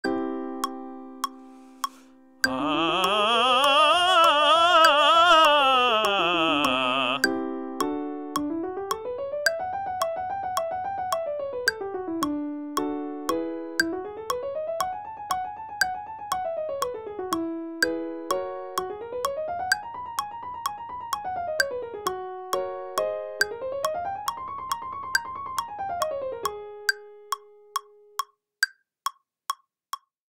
Mixed Voice Vocal Exercise (runs)
Mixed voice vocal exercise:
These enormous runs are designed to increase one's flexibility and agility throughout the mixed registers. It is important to note that perfection is NOT the goal here. The goal is to move as freely and comfortably as possible throughout one's mixed registers, even if some of the notes aren't perfect. ATTENTION: THIS EXERCISE GOES INTO EXTREME RANGES. APPROACH WITH CAUTION AND/OR PROFESSIONAL GUIDANCE.

My ENTIRE Recording Equipment and other recommended things